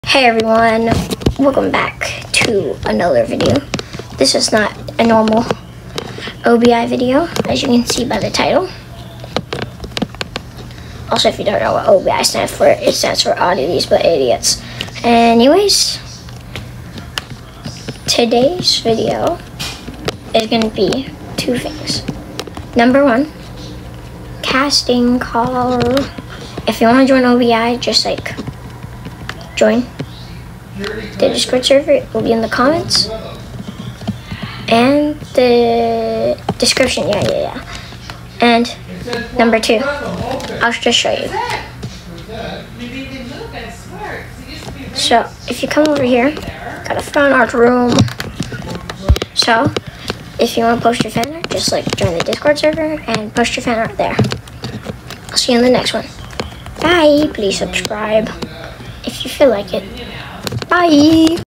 LOOKING FOR ACTORS and we have a fan art room now
I forgo if I said in video but you need mcpe to join the RP and you need discord